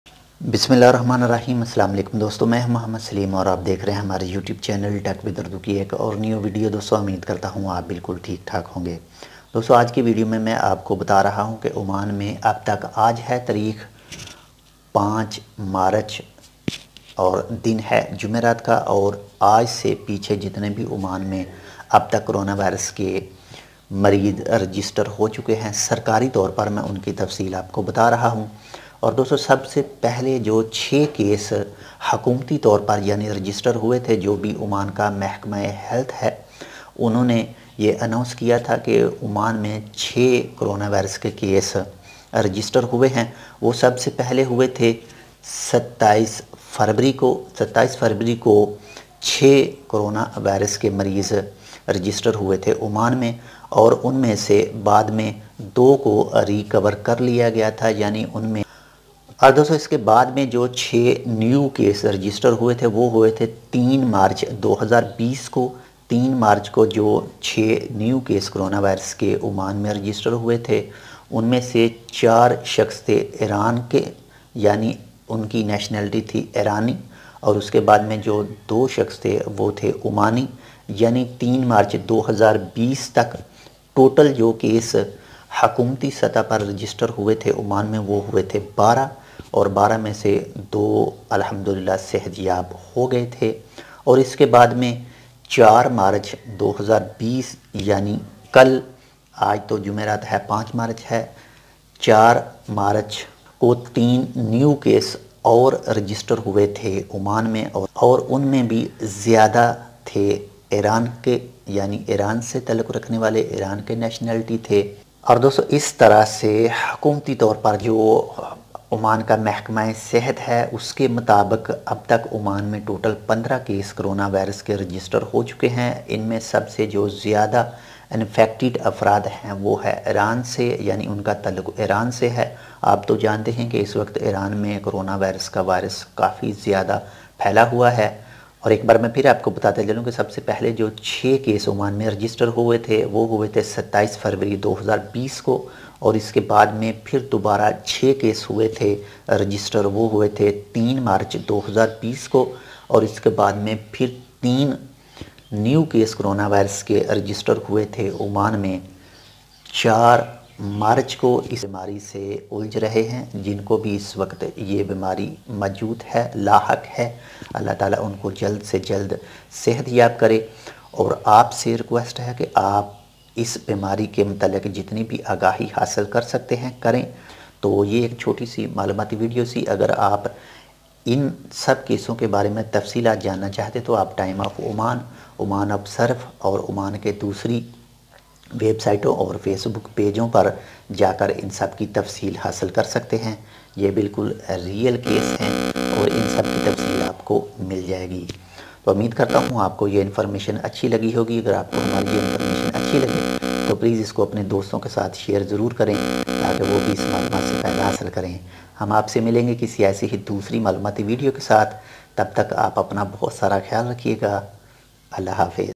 Oman news | How Much cases of COVID 19 in sultanate of oman
ab tak oman muscat main covid 19 ke kitne cases hain oman main in sab ki tafseel is video main,